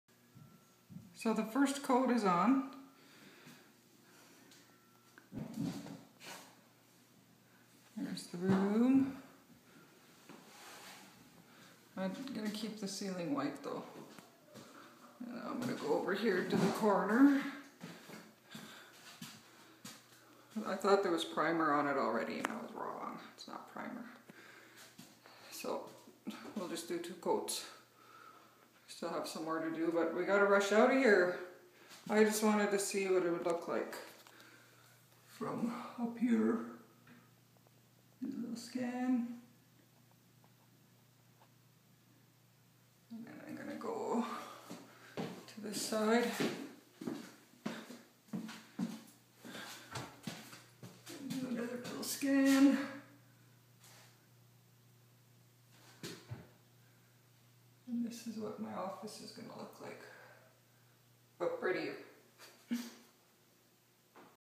Thunderbird Lodge Reno
Summer 2012 making the leap to a new business venture!
Contributing to the betterment of Indigenous people by example. Being self sufficient and making use of what I already have :)